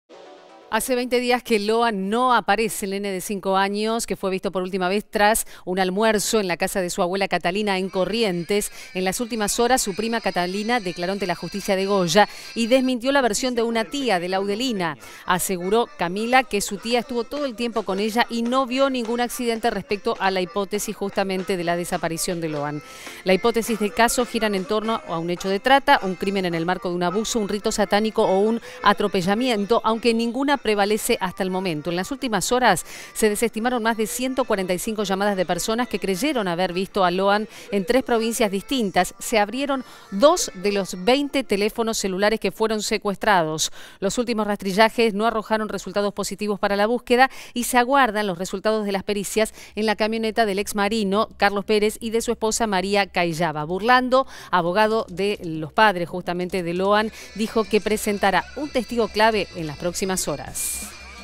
Veinte días sin Loan
En las últimas horas una prima del niño declaró ante la Justicia de Goya y desmintió la versión del accidente. Las hipótesis del caso giran en torno a un hecho de trata, un crimen en el marco de un abuso, un rito satánico o el atropellamiento, aunque ninguna prevalece.